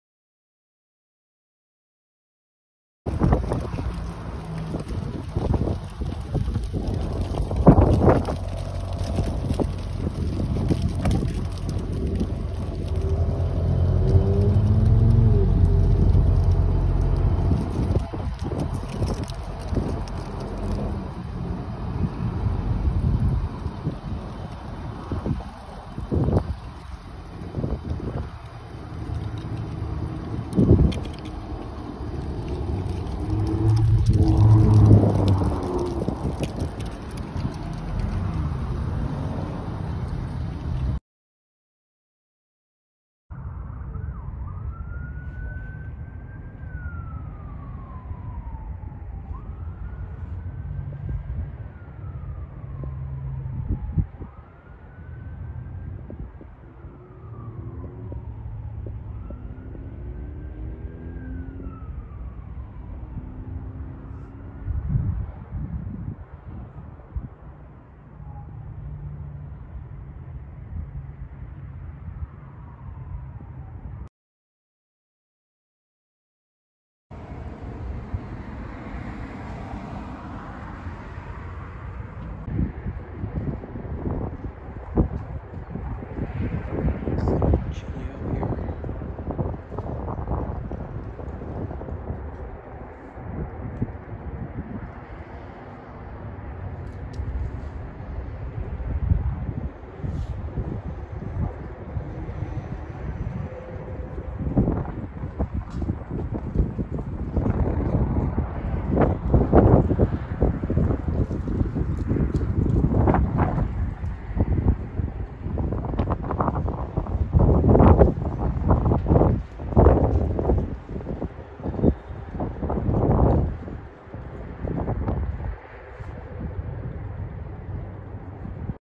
12/12/22- Big time winds and mixed Precipitation/ bitter Windchills into Tuesday
A powerful storm system brought strong winds throughout New Mexico and sweeping across the nation. 35 to 40 mph wind gusts with higher gusts everywhere else, rain, snow and graupel all hitting the Metro with also a big cooldown that will really bring a chill to Albuquerque, temps for the next 7 days and into next week will be in the upper 30’s and lower 40’s for highs, and we are even watching another big time storm system come through possibly early next weekend and the week before Christmas, which will just reestablish the cold temps, so it doesn’t look like we’ll pull out of this cold snap until possibly next year🥶.